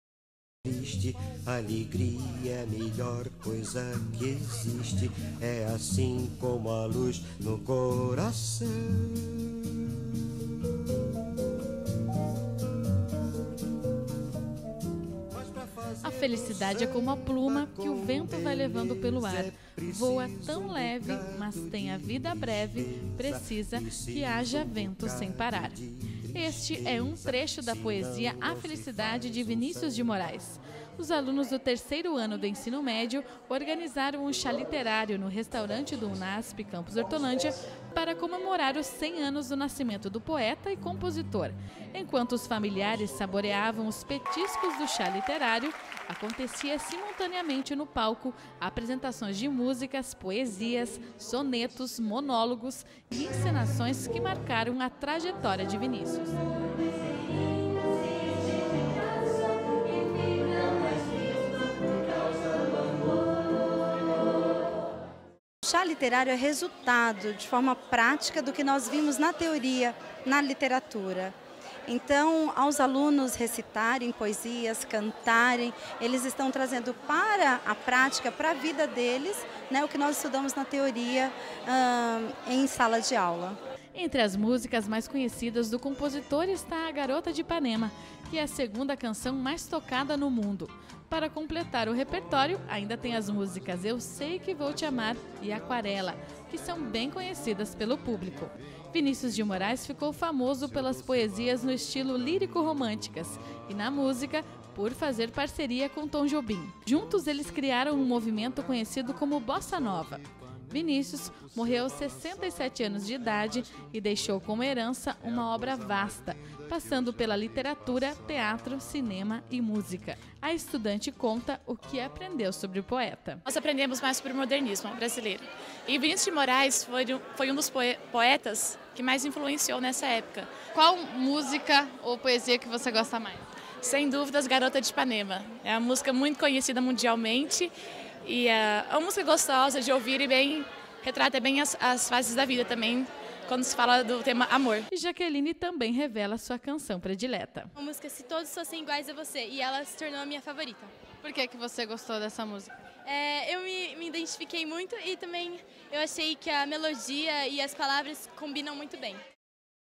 Chá literário faz homenagem ao poeta Vinicius de Morais
No dia 19 de outubro, Vinicius de Moraes, um dos maiores poetas e compositores da música brasileira, completaria 100 anos. Um chá literário prestou uma homenagem ao poeta da bossa nova.